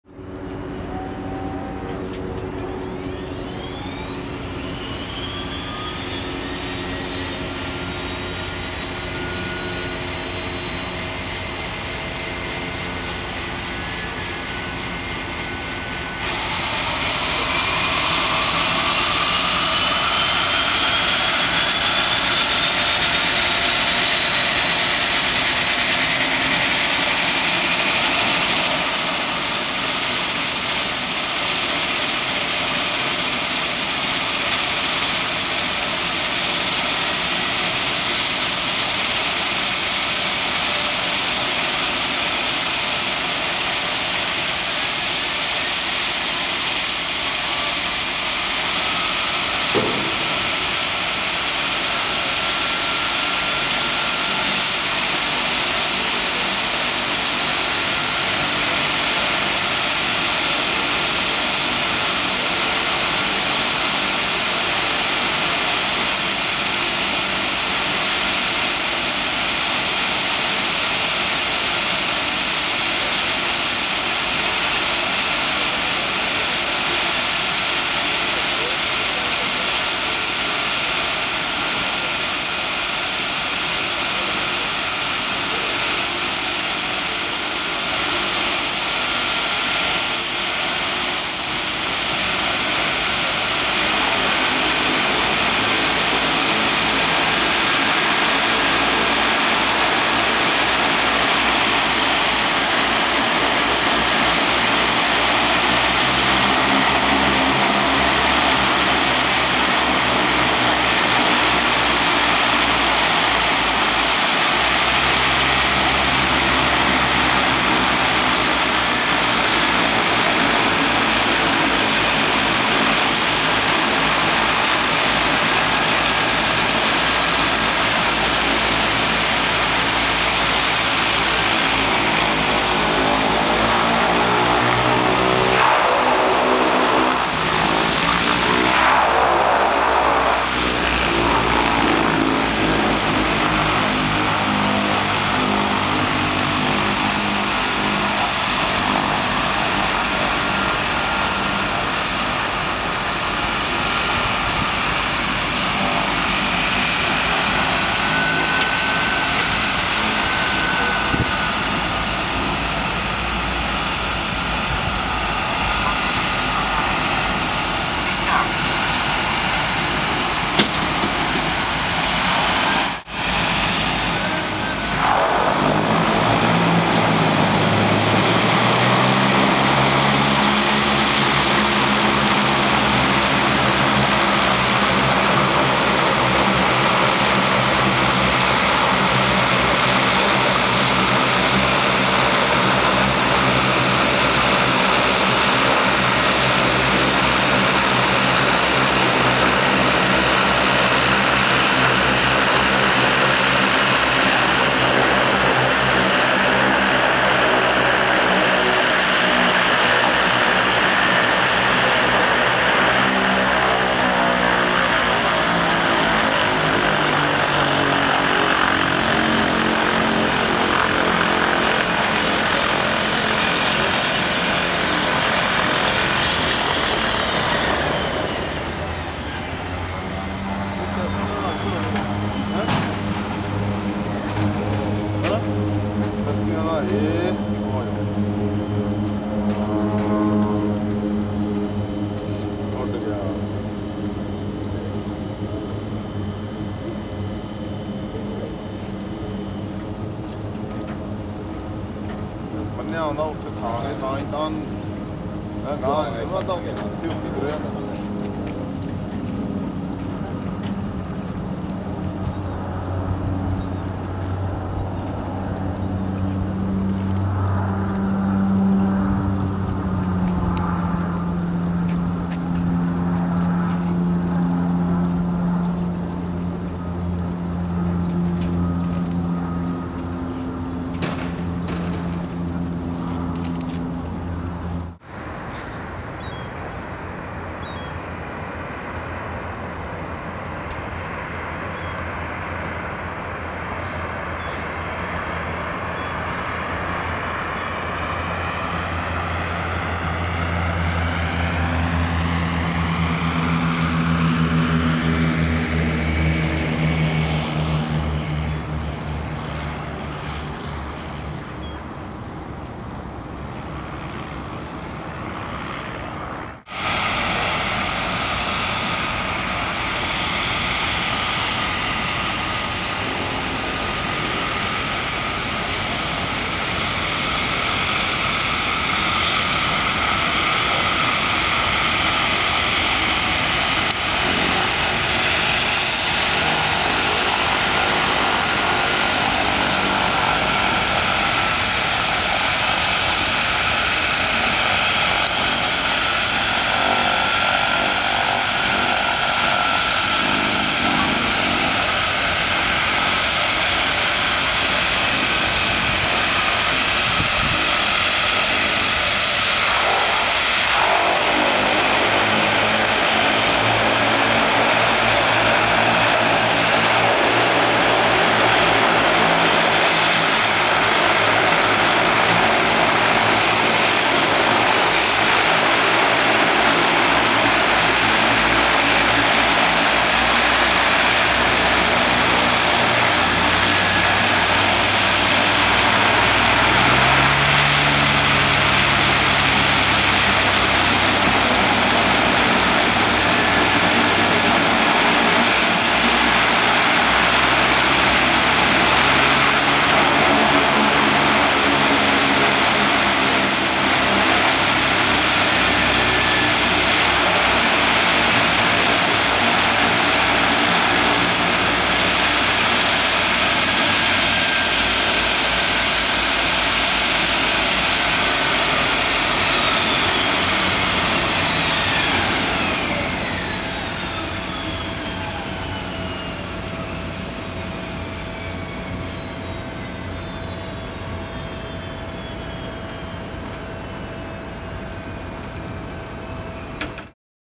DHC-6 Twin Otter Engine Start Up and Takeoff
West Coast Air (Harbour Air) de Havilland Canada DHC-6 Twin Otter Seaplane C-GQKN is seen at the Vancouver Harbour Water Airport CYHC in Vancouver, British Columbia. This video includes the turboprop engine startup, takeoff, landing, and the engine shutdown.